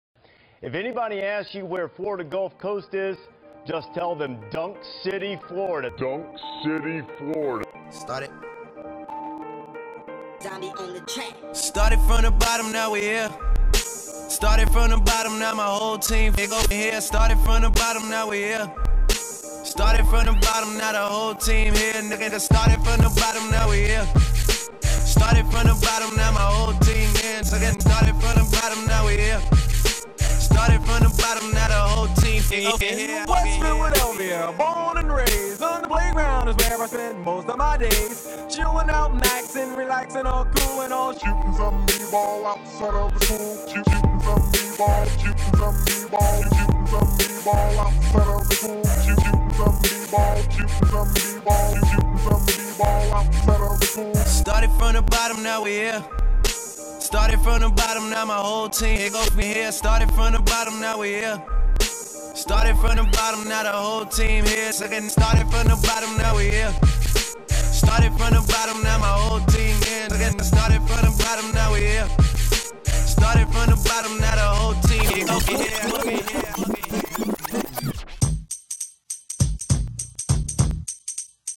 FGCU Basket Ball HYPE
FGCU students showing eagle pride in Philadelphia ! These students packed on a bus for a 22 hour adventure to support our team! GO EAGLES!

Find more at Facebook.com/gphotofl